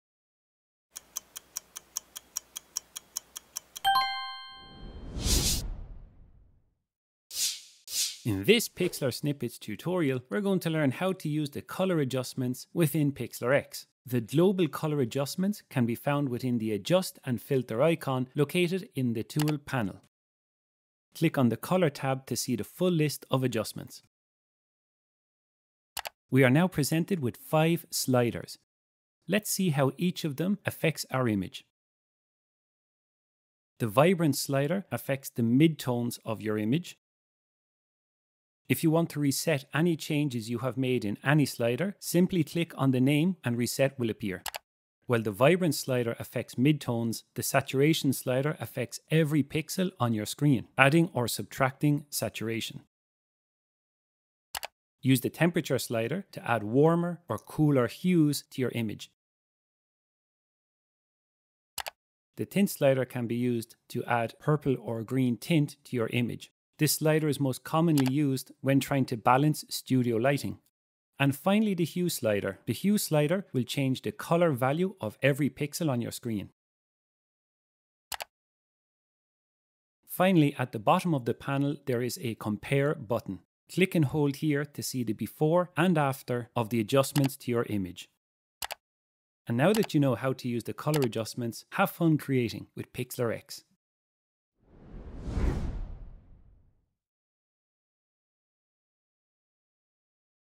Quick & Easy Series: Color Adjustments in Pixlr X
In today's Quick & Easy tutorial, learn how to use the color adjustment tools in Pixlr X to enhance your images' color and tone💫

✨AI-powered online photo editor
✨Quick & Easy photo editor
✨Works on all browsers + devices